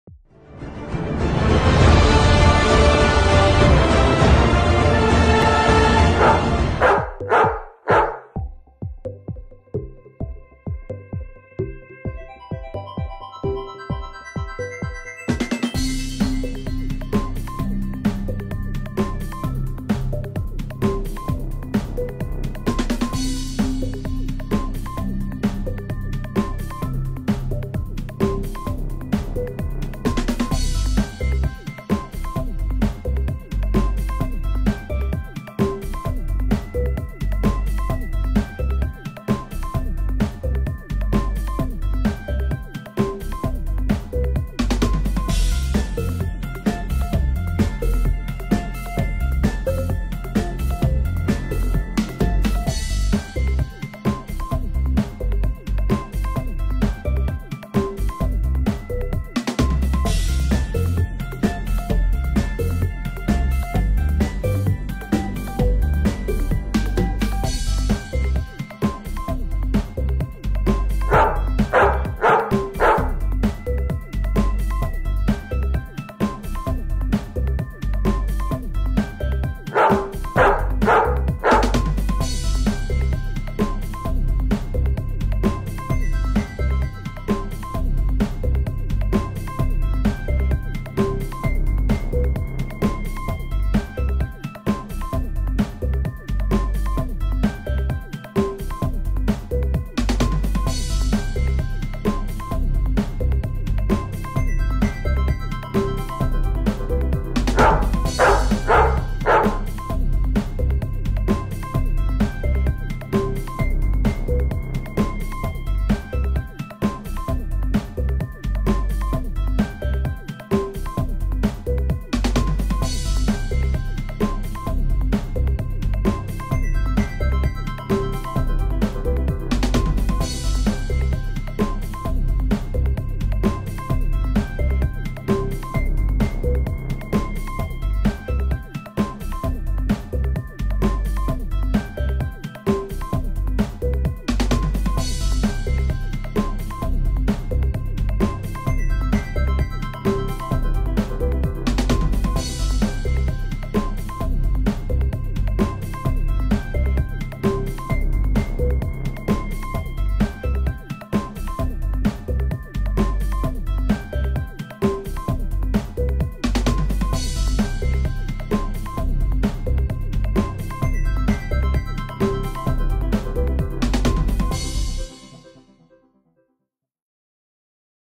Meet Charlie having food with daddy
My Charlie dog